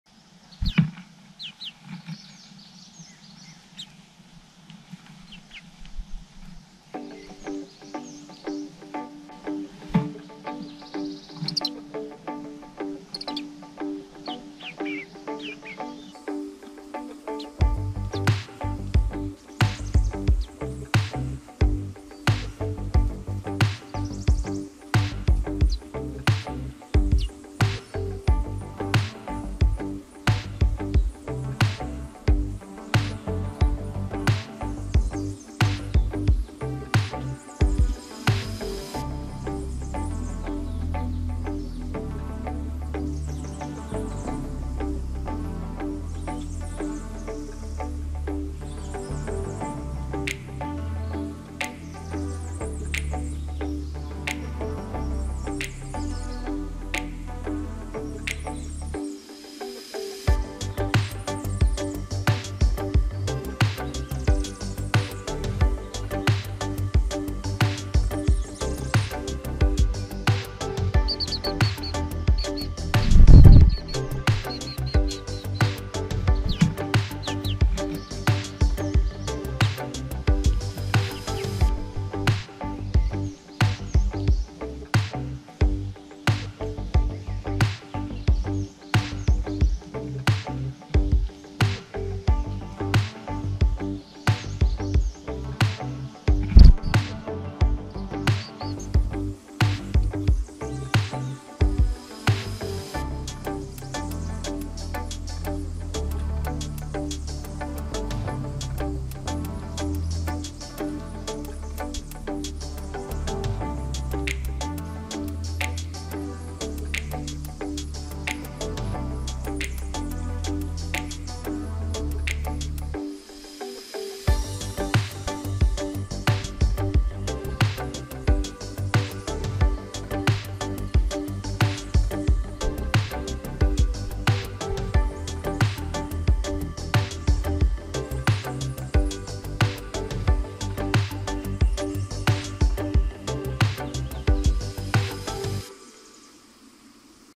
Cận Cảnh Tổ Chim Sẻ Thay Phiên Nhau Chăm Đàn Con Trên Cây Thông
Link Kênh Hieu NightSky Anh Bủm : @HieuNightSky
Link Kênh Shorts Hieu NightSky  : @NhacNoCopyright